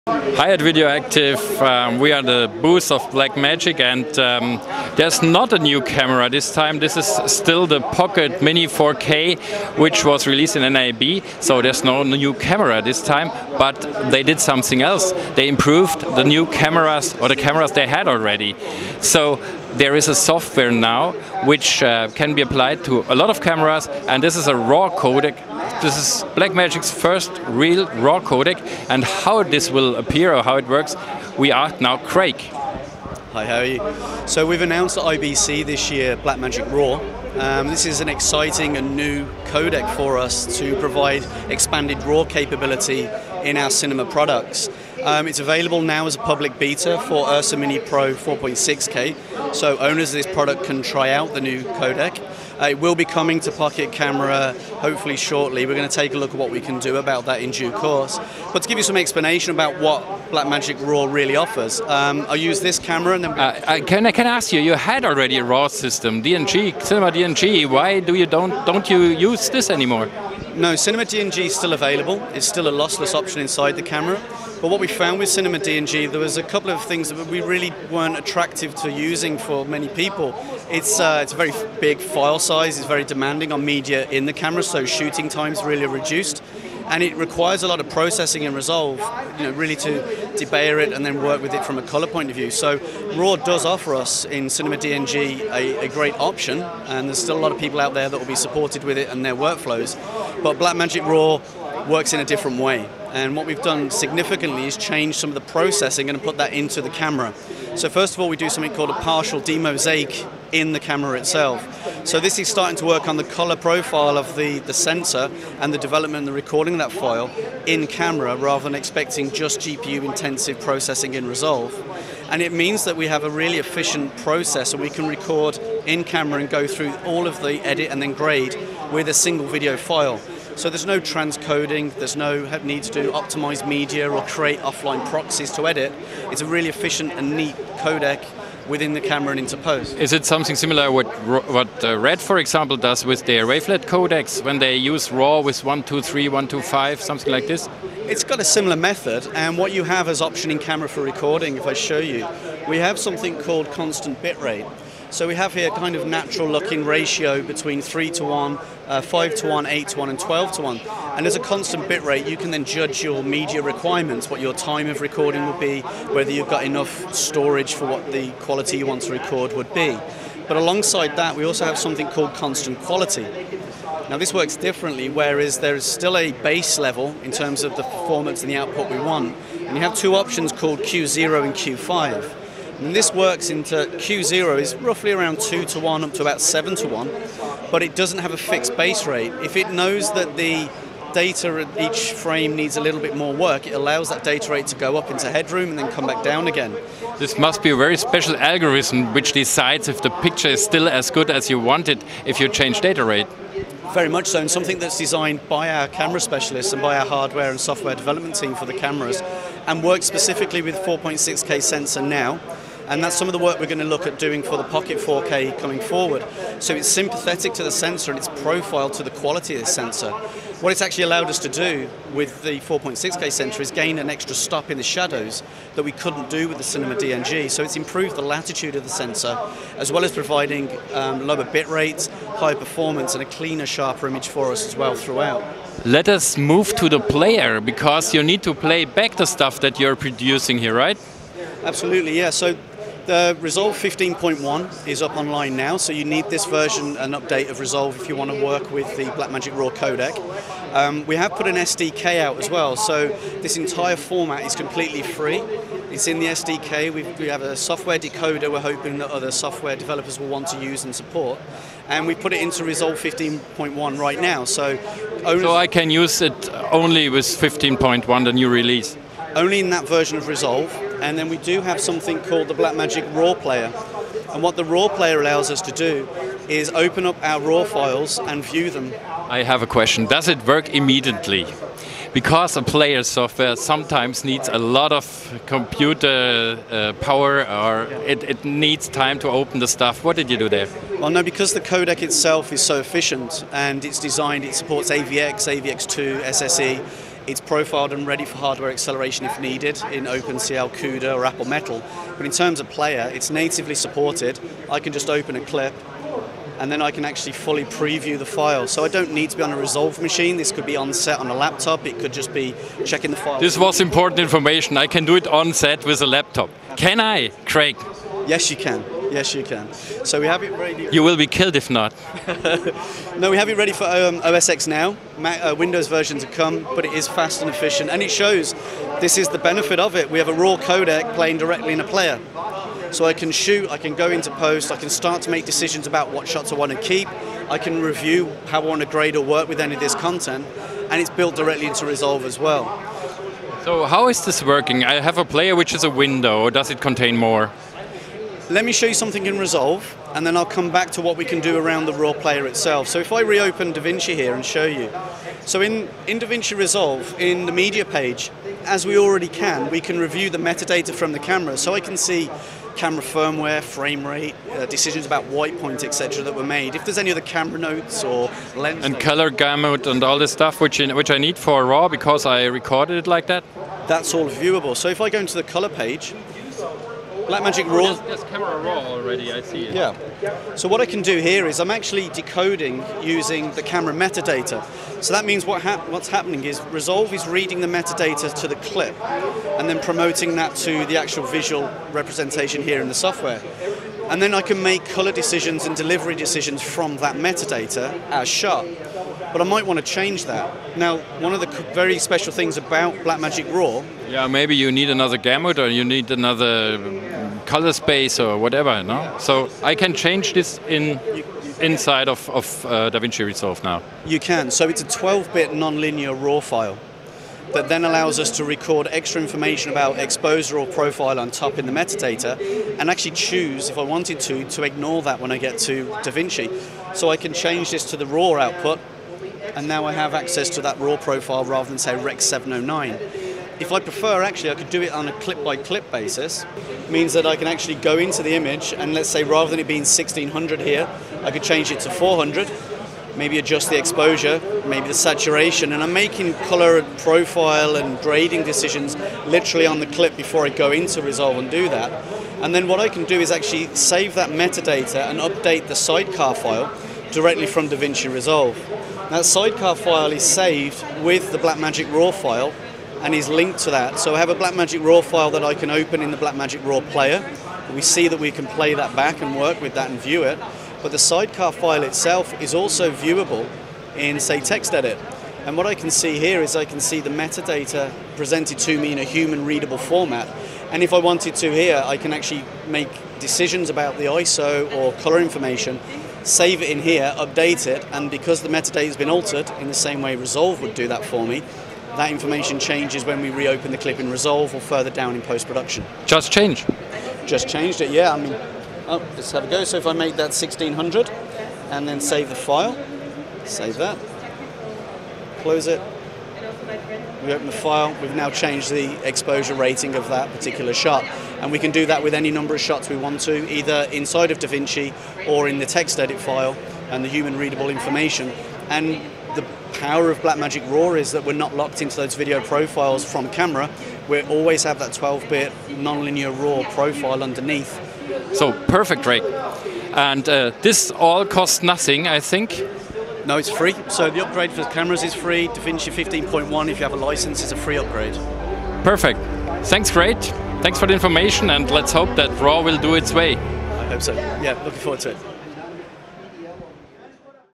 IBC 2018: Blackmagic RAW - das kann der neue Codec
Nun hat auch Blackmagic als einer der letzten Profi-Camcorderhersteller einen Raw-Aufzeichnungsmodus vorgestellt. Was er kann und warum er garnicht so RAW ist, erklärt  Produktspezialist Craig Heffernan in diesem Video. Er zeigt auch die Decodierung der Daten in einer BM-Playersoftware und in Davinci-Resolve 15.1.

erreichen.